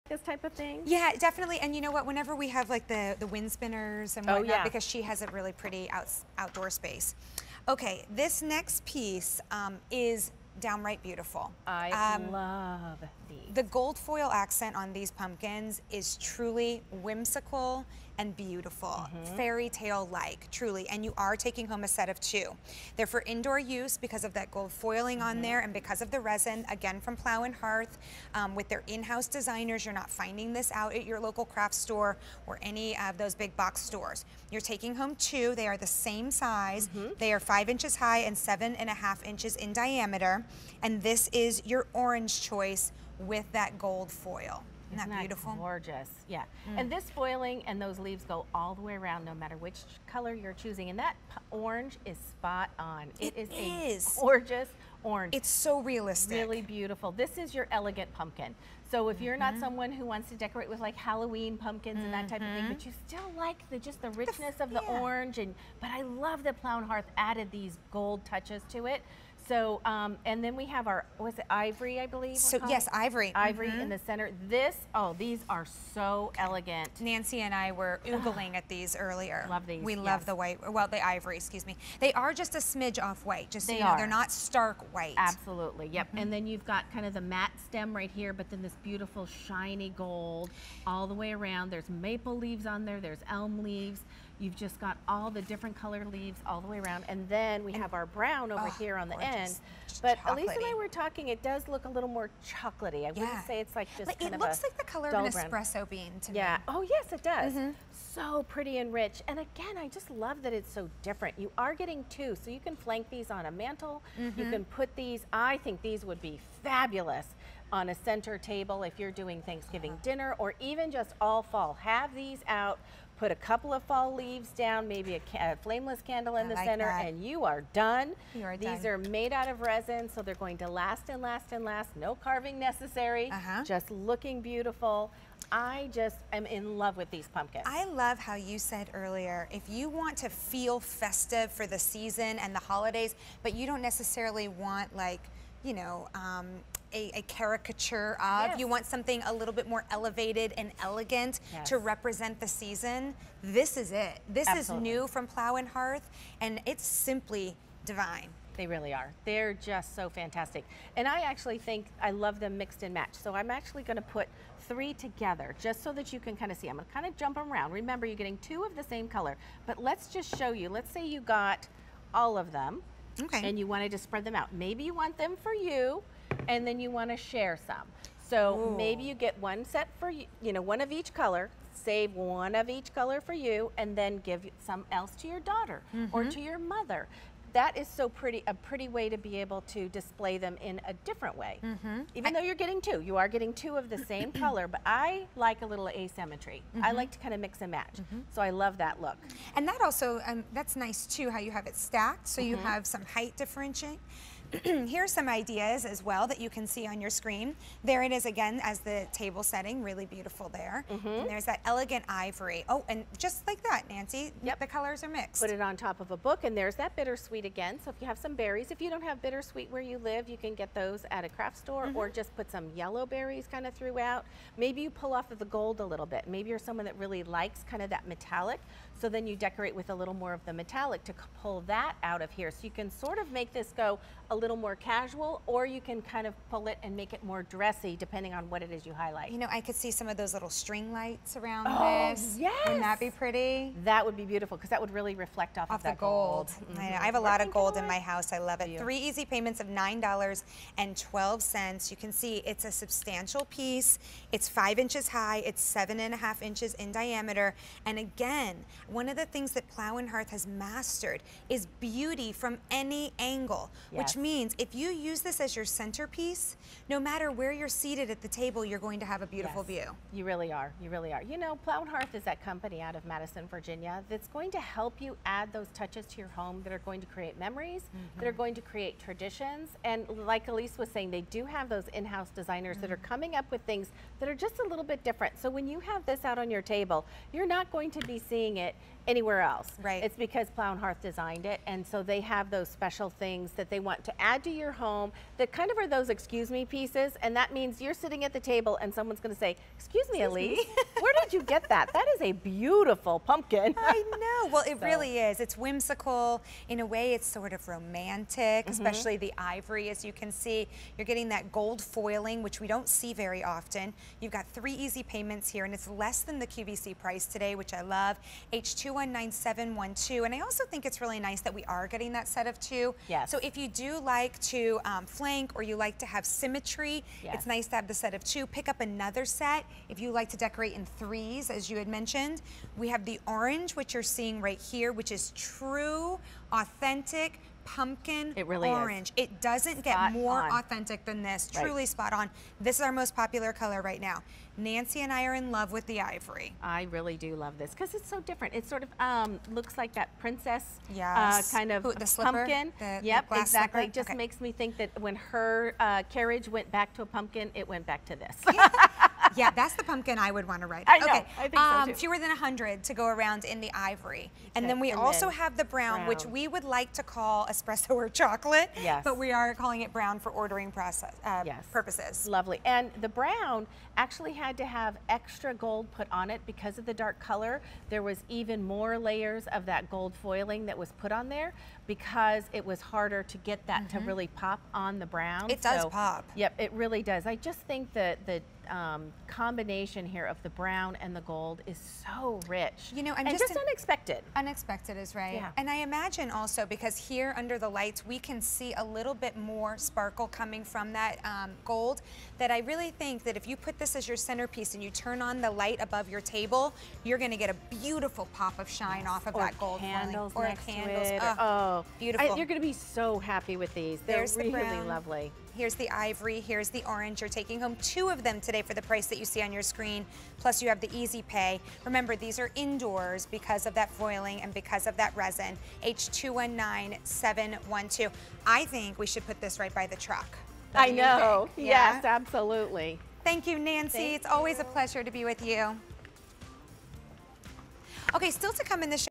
Plow & Hearth Set of 2 Resin Pumpkins with Gold Foil Accents on QVC
Plow & Hearth Set of 2 Resin Pumpkins with Gold Foil Accents

Nothing says autumn like the changing colors of the leaves and pumpkin picking. Get a taste of both with these resin figurines when you add them to your harvest display. From Plow & Hearth.